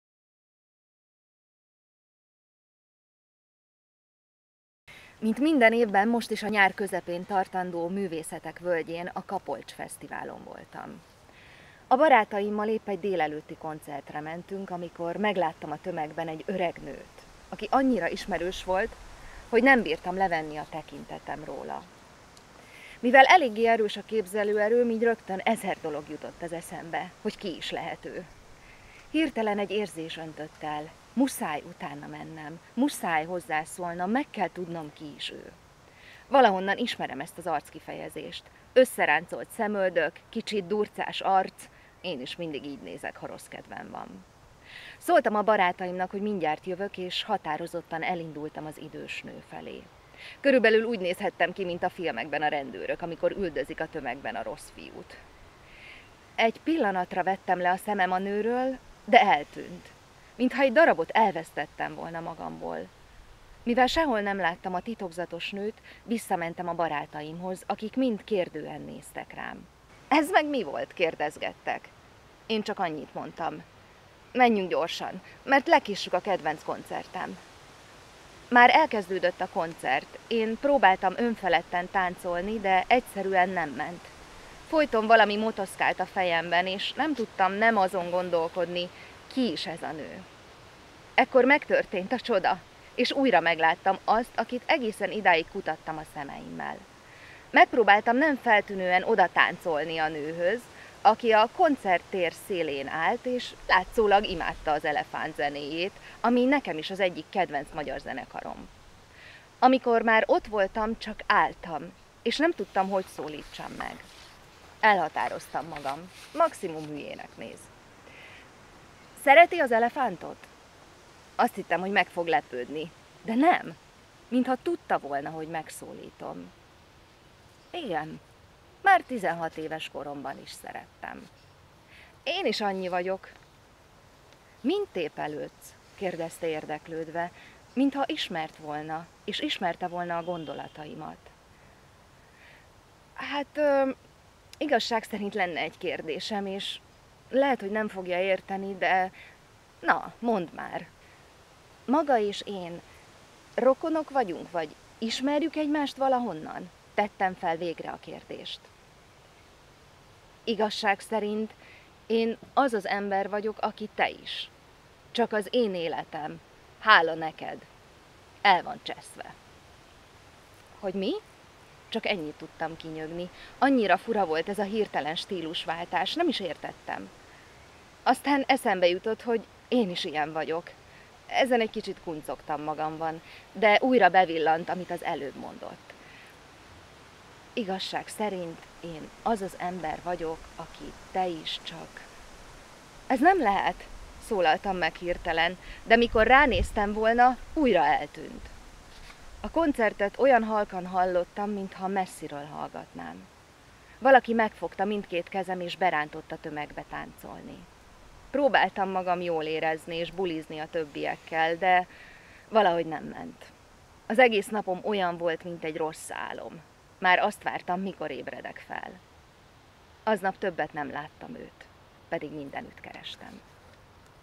Angelus Anna: Találkozás önmagammal (részlet)
A novella a Gyerekfilm Akadémia és a Pagony kreatív írótanfolyamán készült.
Előadják: Bozó Andrea